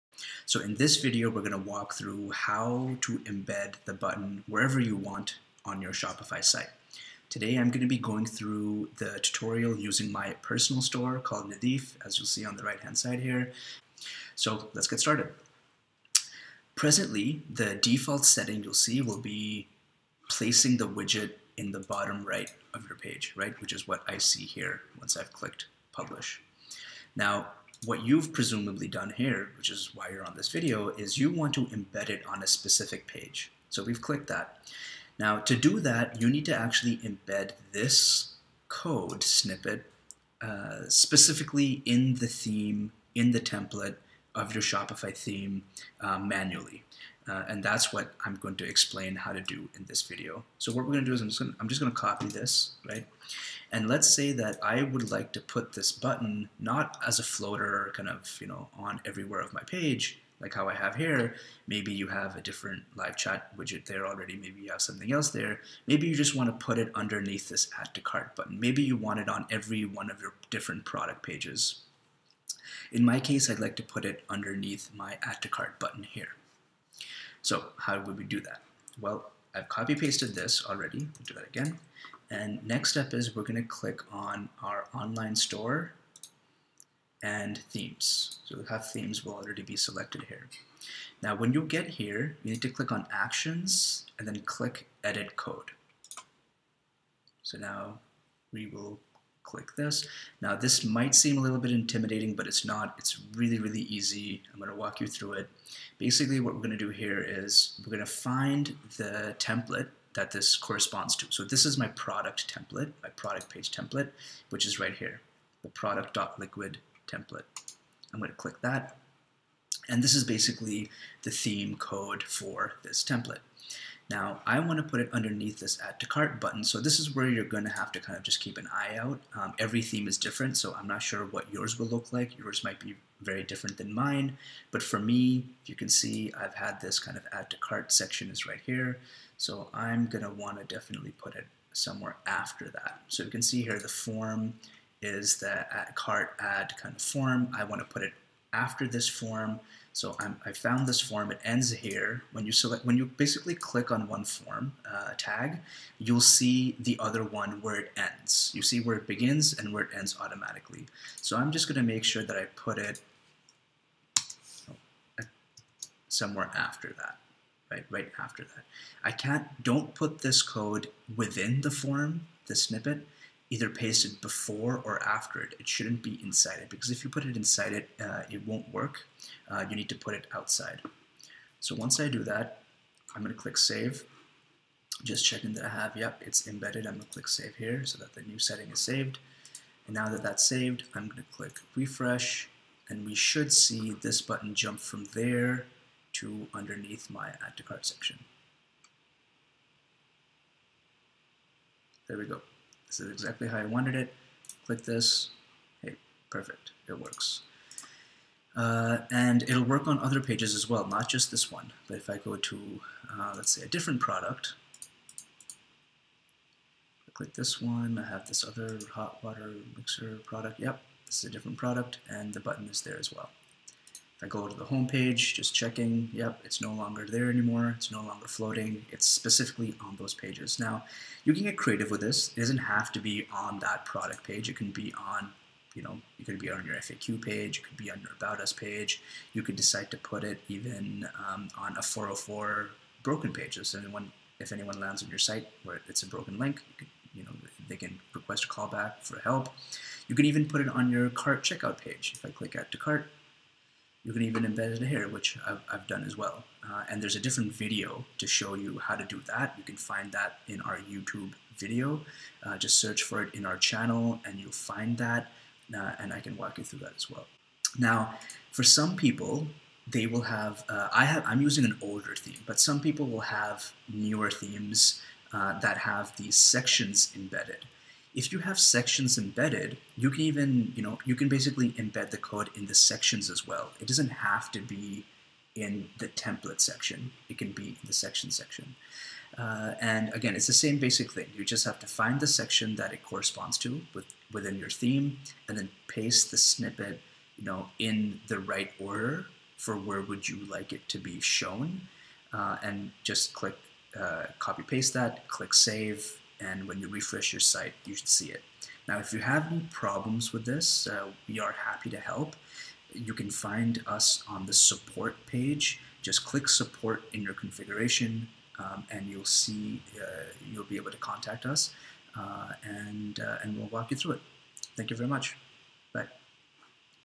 Embedding Raven into Specific Pages - How To Video
In this video I go through how to add the Raven Call Back button to your Shopify website.

For the video on how to embed the Raven snippet it into the Cart details page of your website as shown later in this video

You have two placement options:

1. Floating button on any of the four corners of your website (no code snippet pasting required for this)

2. Embedded on specific pages in specific locations (the topic for this video)